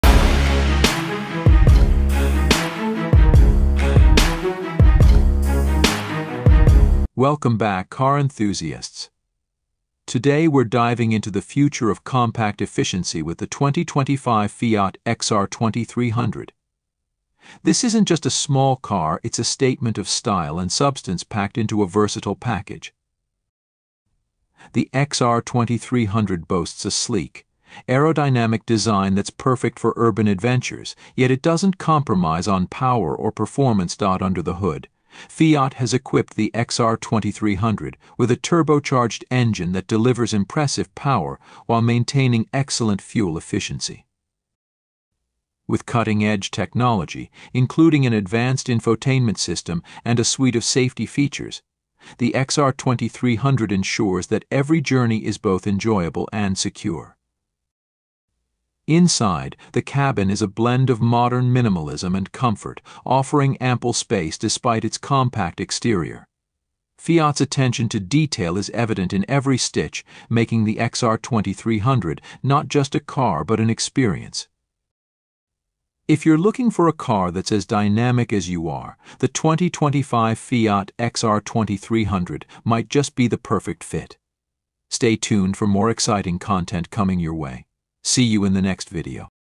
2025 Fiat XR | Compact Luxury Meets Cutting-Edge Technology
2025 Fiat XR | Compact Luxury Meets Cutting-Edge Technology

Introducing the 2025 Fiat XR – a revolutionary step forward in automotive design and performance! This sleek and stylish vehicle combines cutting-edge technology with Fiat's signature Italian flair, delivering an unparalleled driving experience. From its powerful engine to its state-of-the-art interior, the Fiat XR is designed to excite and inspire. Join us as we dive deep into the features, specs, and what makes this model stand out in the competitive automotive market. Don't miss out on this comprehensive review of the 2025 Fiat XR.